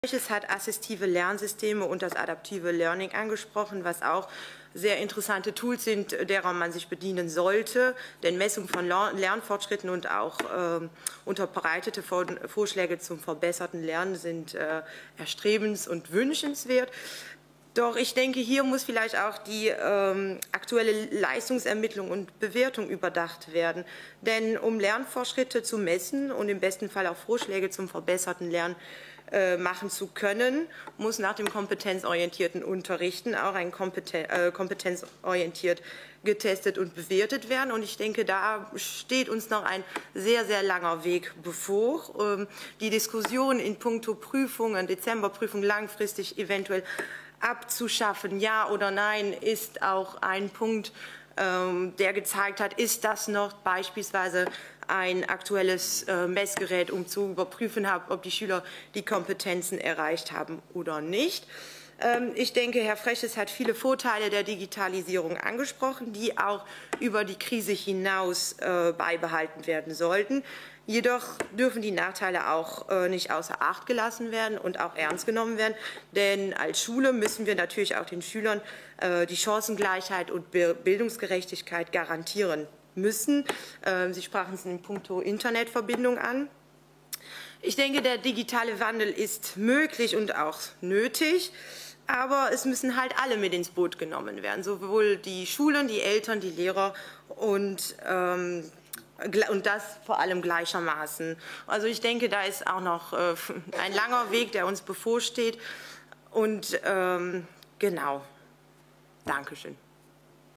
Stephanie Pauels "Wir müssen Schule neu denken" | CSP Ostbelgien
Was sollten unsere Kinder und Jugendlichen heute lernen und wie bereitet die Schule sie am besten auf die Zukunft vor? Für unsere PDG-Abgeordnete Stephanie Pauels steht fest, dass neue Schul- und Unterrichtskonzepte erarbeitet werden müssen. In ihren Augen sollten neue Kompetenzen in den Fokus gerückt, Leistungsermittlung und Bewertung der Schüler*innen überdacht und der digitale Wandel in den Schulen gleichberechtigt für alle eingeführt werden. Zusammen mit der Jungen Mitte möchte Pauels ebenfalls das bereits 2016 eingereichte Schulkonzept zur Bürgerkunde erneut aufgreifen.

"In einer Zeit, in der sich die Gesellschaft so schnell wandelt, wird es immer wichtiger, die Schüler*innen bestmöglich und verantwortungsbewusst auf die Zukunft vorzubereiten. Kompetenzen wie Selbständigkeit, Flexibilität, eigenverantwortliches Handeln und kritisches Hinterfragen hätten längst in den Mittelpunkt des schulischen Lernprozesses gerückt werden müssen. Außerdem sollten lebensnahe Themen aus dem Alltag der Schüler*innen, wie beispielsweise das Mietrecht, Steuererklärungen, usw. fester Bestandteil des Lehrplans werden. Um dieses Ziel zu erreichen, braucht es neue Schul- und Unterrichtskonzepte!", so Steffi.

"Die aktuelle Leistungsermittlung und Bewertung sollte überdacht werden! Denn um Lernfortschritte zu messen und im besten Fall Vorschläge zum verbesserten Lernen machen zu können, muss nach dem kompetenzorientierten Unterrichten auch kompetenzorientiert getestet und bewertet werden - und da steht uns noch ein langer Weg bevor!". So dränge sich die Frage auf, ob Dezemberprüfungen noch ein zeitgemäßes Messgerät seien, um die Kompetenzen der Schüler*innen zu testen.

Darüber hinaus stelle sie fest, dass die Digitalisierung zwar viele Vorteile mit sich bringe, die über die Krise hinaus beibehalten werden sollten, jedoch dürften die Nachteile nicht außer Acht gelassen werden. „Als Schule müssen wir den Schüler*innen Chancengleichheit und Bildungsgerechtigkeit  garantieren. Hier spielen Faktoren wie entsprechendes Material, eine gute Internetverbindung, usw. eine wichtige Rolle. Wem nützt ein Laptop ohne gute Internetverbindung oder andersrum?", so Pauels.

Die Junge Mitte entwickelte bereits 2016 einen Konzeptvorschlag, der dem damaligen Schulminister überreicht wurde. Das Schulkonzept befasste sich zum einen mit der Bürgerkunde, die tagesaktuelle und politische Themen, das belgische Staatssystem und die Verwaltungsgliederung sowie das Steuer- und Vertragswesen aufgreifen sollte. Zum anderen forderte das Konzept eine modernere Gestaltung des Religions- und Sportunterrichts. So lautet ein Vorschlag, im Sportunterricht der Sekundarschule alljährlich einen Erste-Hilfe-Kurs vorzusehen. „Wir arbeiten zurzeit gemeinsam an einer Weiterentwicklung des bestehenden Konzepts. Denn zum Beispiel das Thema ‚Medienkompetenz‘ wird immer wichtiger, unter anderem sollten der Umgang mit Social Media, die möglichen Auswirkungen des Social Media Konsums auf die physische und psychische Gesundheit von Kindern und Jugendlichen sowie das kritische Lesen und Untersuchen von Quellen, um Fake News zu erkennen, so schnell wie möglich Teil der Lehrpläne sein“, so Pauels und die Junge Mitte Co-Vorsitzenden Lukas Teller und Shirin Tjandra abschließend.